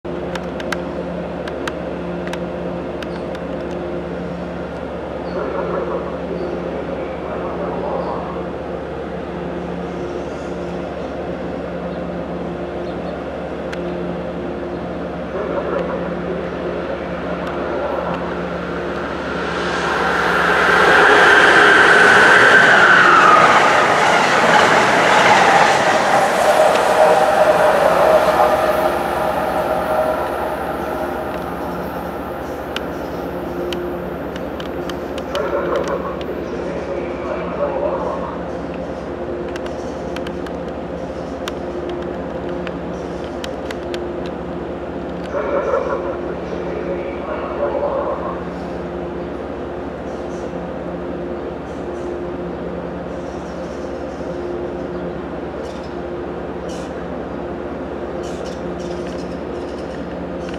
Amtrak northeast regional readville
Rail fanning at readville station, amtrak northeast regional 66, going through readville station, heading to boston south station.